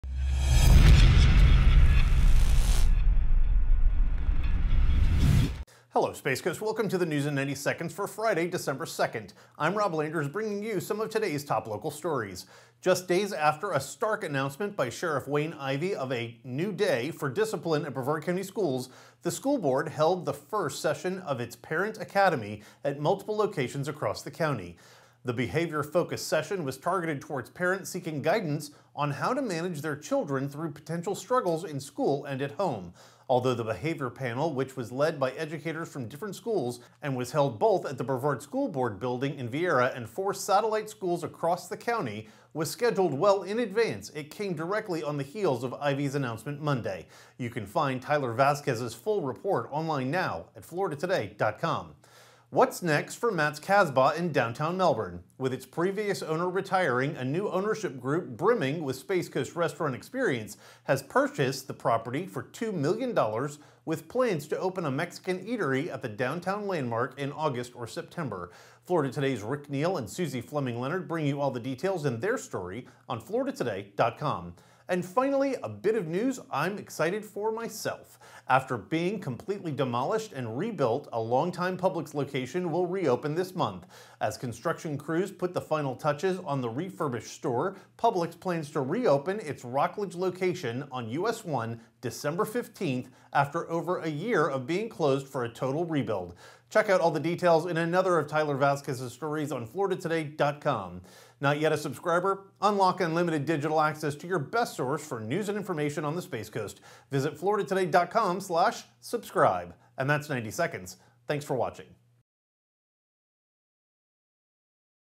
News in 90 Seconds: Brevard 'Parent Academy', Matt's Casbah and Rockledge Publix
FLORIDA TODAY's Rob Landers brings you some of today's top local stories on the News in 90 Seconds.